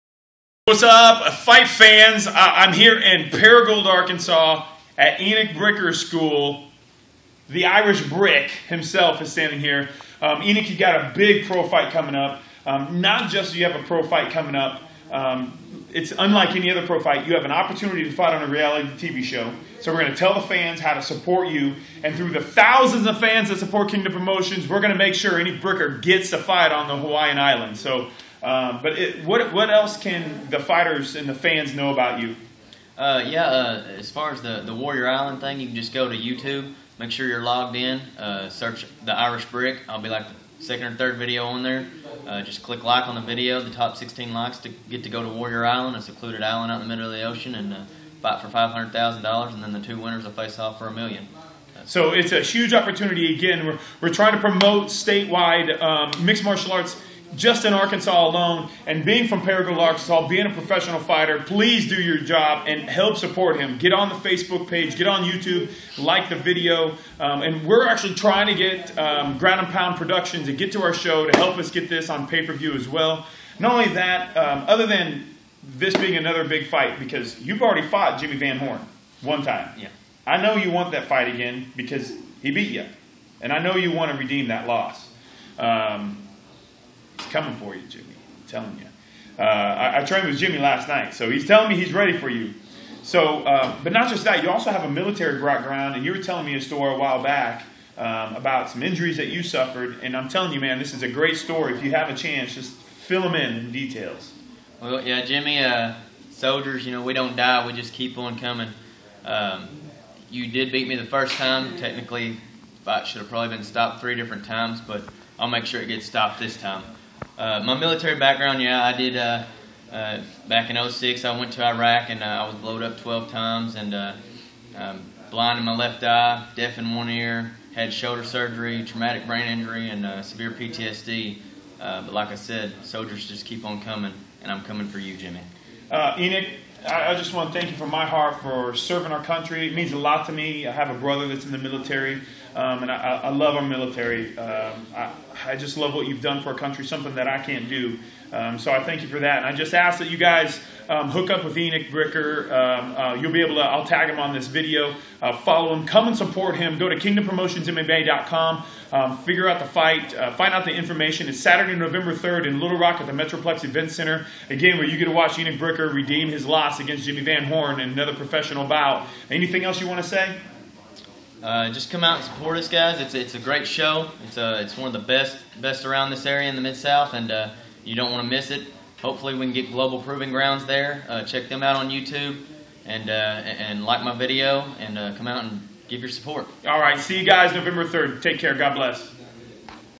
Mma interview w pro Enoch Bricker "Irish brick"
Enoch Bricker says he's coming for Jimmy VanHorn. A military veteran and professional mixed martial arts fighter, Enoch has a lot riding on this upcoming fight. He has an opportunity to fight on a new reality tv show. So please find his youtube video of his Global Proving Ground audition and like it and share it. sat. November 3rd